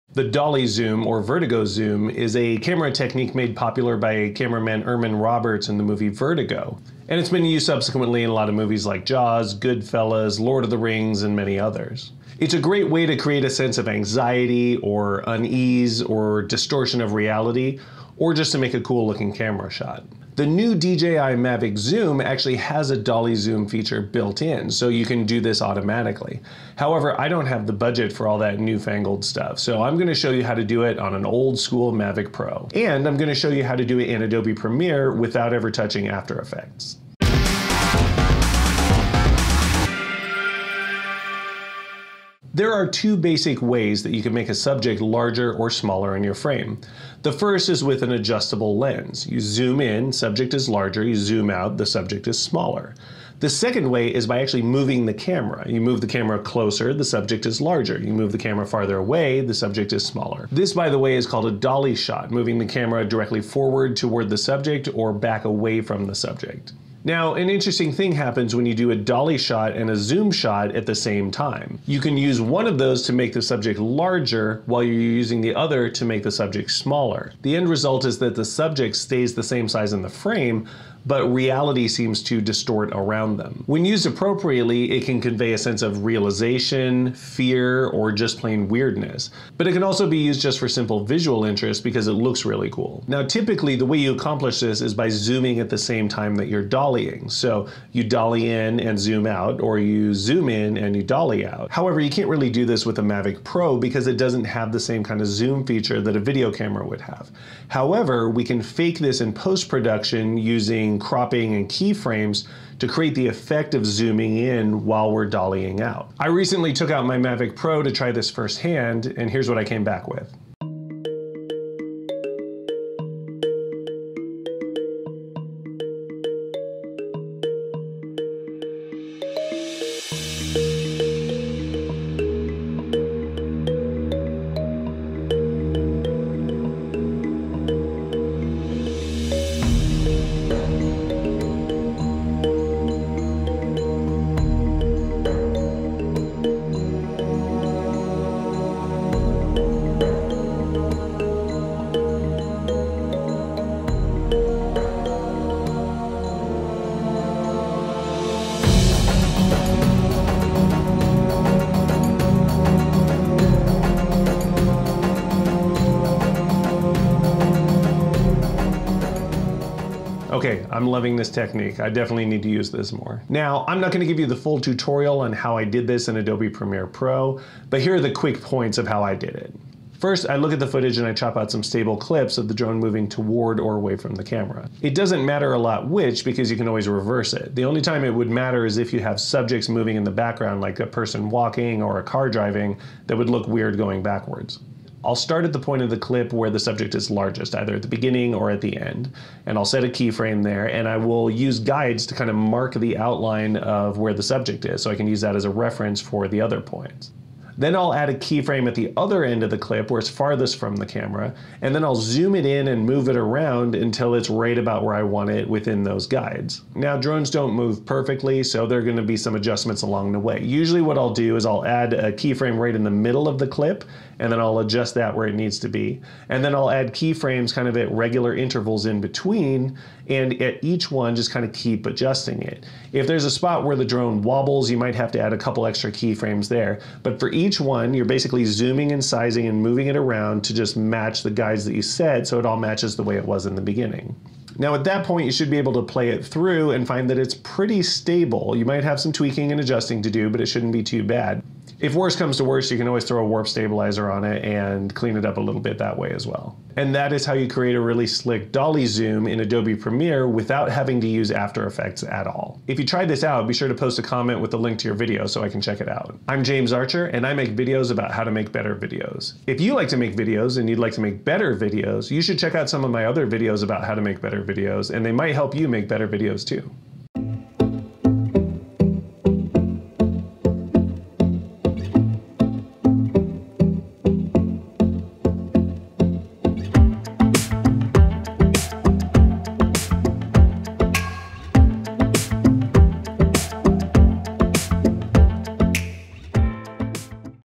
MIND-BENDING Vertigo effect dolly zoom with your DRONE (no After Effects)
How to do a dolly zoom (Vertigo zoom) with your drone, without requiring After Effects!

The "dolly zoom" or "Vertigo zoom" is a camera effect made famous by cameraman Irmin Roberts in the movie "Vertigo," and used prominently in other films like Jaws, Goodfellas,  Lord of the Rings, and many other famous films. It's a great way to convey a sense of anxiety, uneasiness, or distortion of reality, or sometimes just to make a cool-looking shot.

The new DJI Mavic 2 Zoom has a built in dolly zoom feature that lets you do this automatically. However, I don't have the budget for all that newfangled stuff, so I'm going to show you how to do it on the original, old-school Mavic Pro. And I'm going to show you how to do it in Adobe Premiere, without needing to use After Effects.

~94